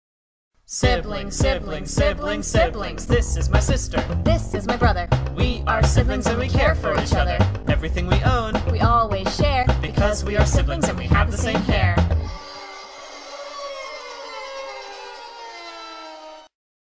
Siblings GC Meme || Gacha Club Meme
[MESSAGE]
I saw a little animatic of this video in my recommended one day, and I COULD NOT get this sound out of my head. It's just got that special feel, you know?

[STATS]
Views: 931,835